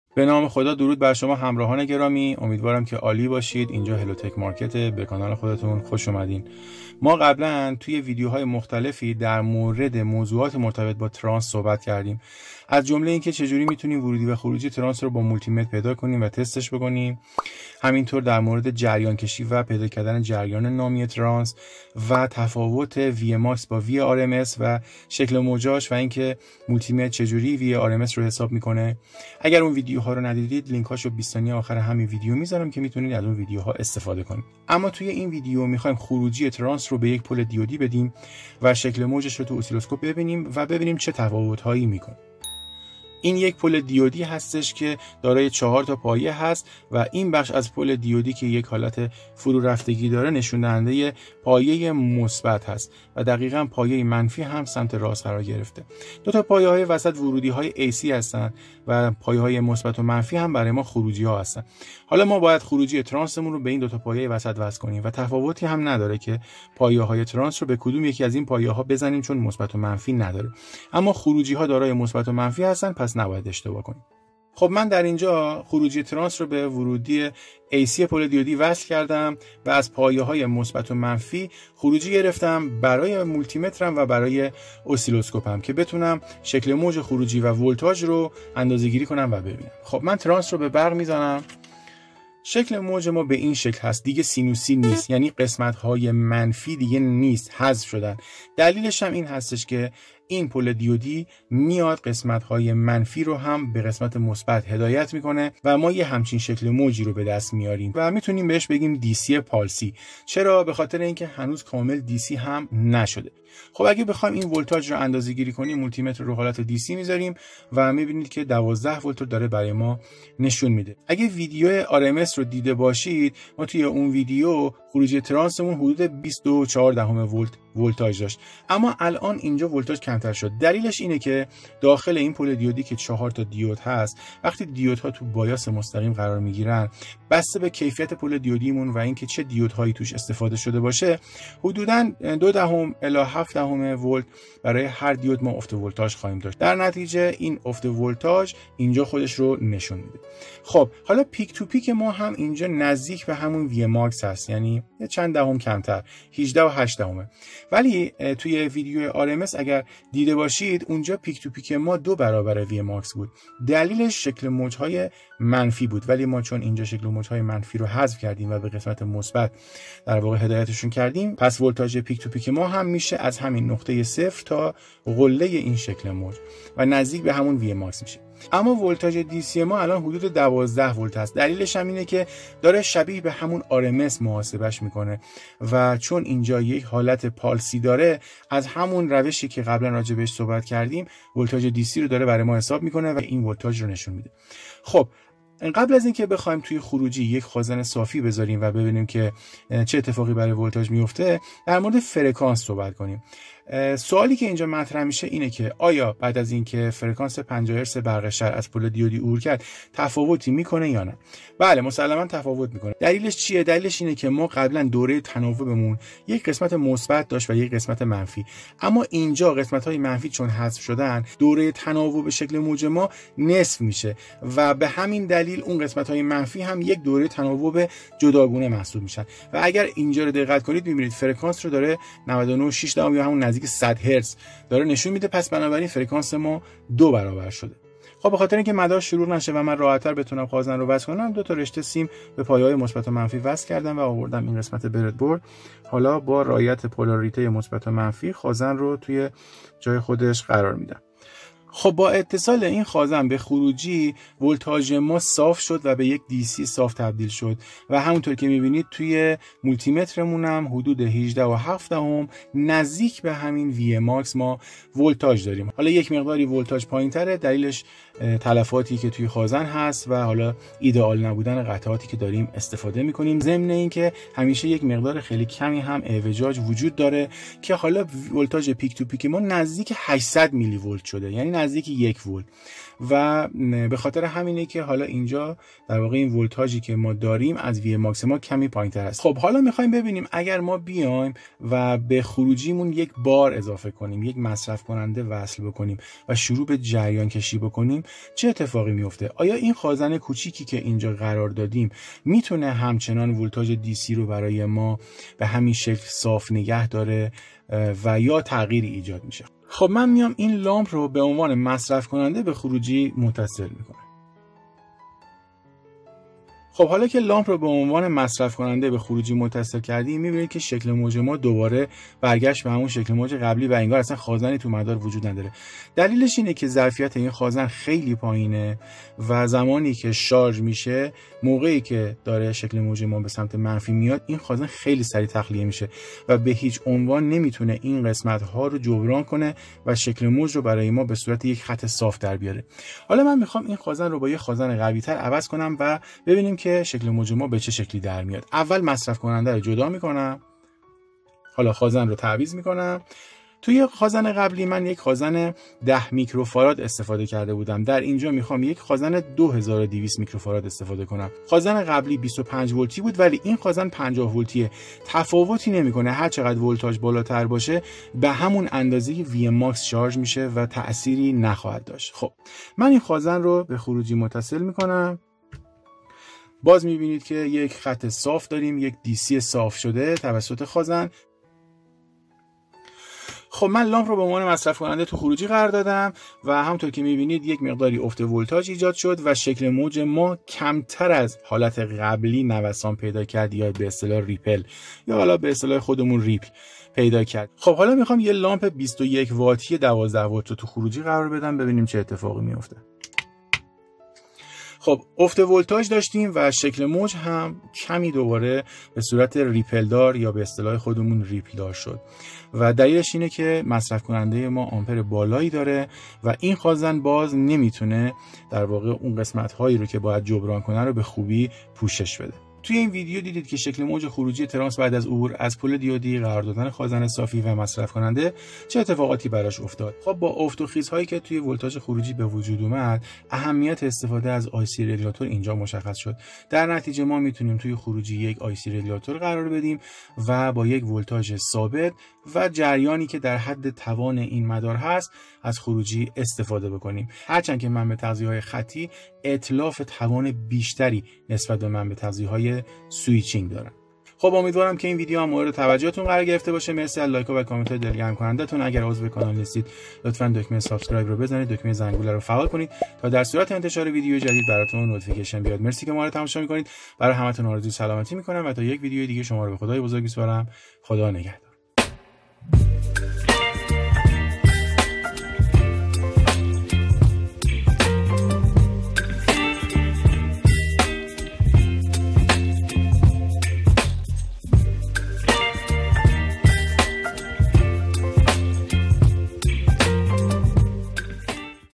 شکل موج ولتاژ ترانس بعد از پل دیودی: خازن صافی چه تاثیری داره؟
شکل موج ولتاژ ای سی بعد از پل دیودی: خازن صافی چه تاثیری داره: فرکانس چرا دوبرابر شد